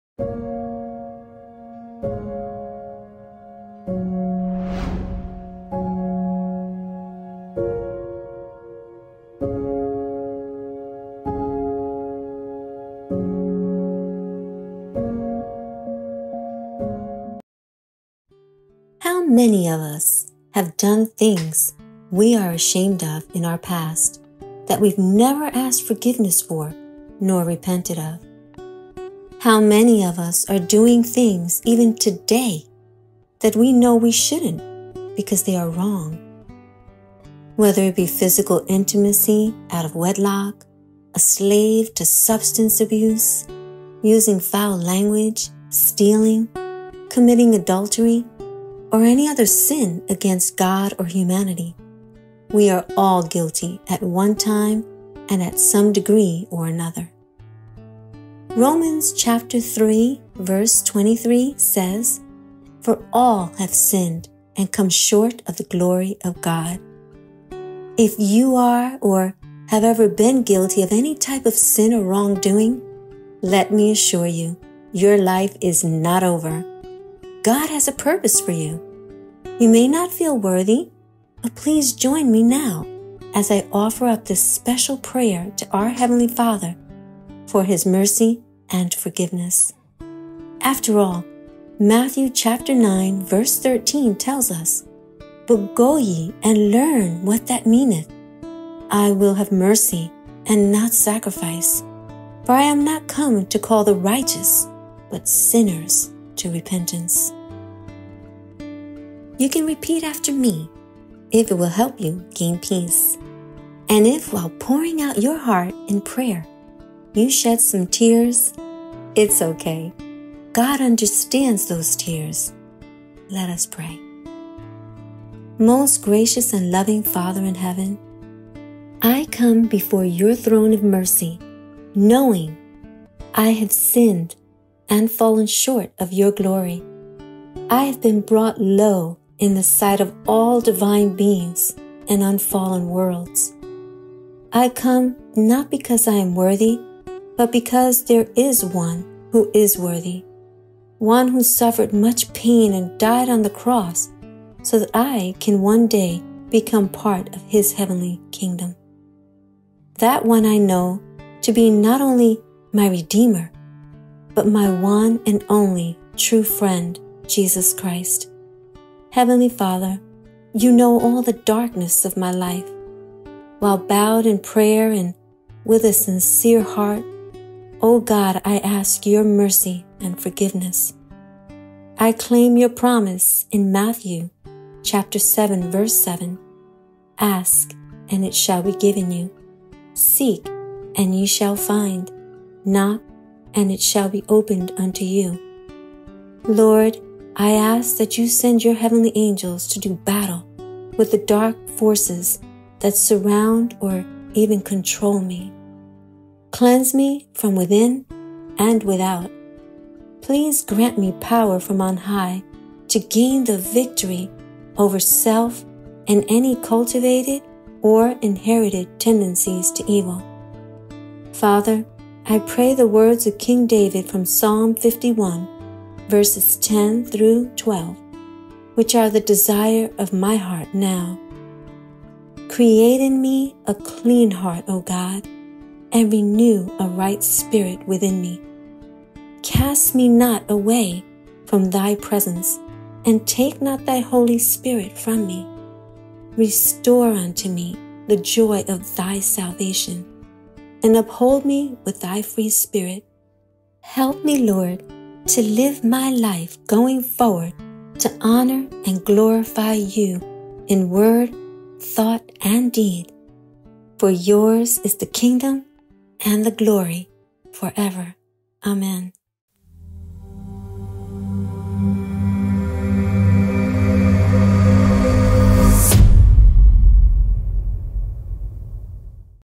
Prayer for Mercy and Forgiveness - The Bible Says
Welcome to a new video of The Bible Says. This time we invite you to pray from the depths of your heart to find forgiveness and mercy from God. No one has ever fallen so low that God cannot rescue them, He has made provision for any fault we have committed.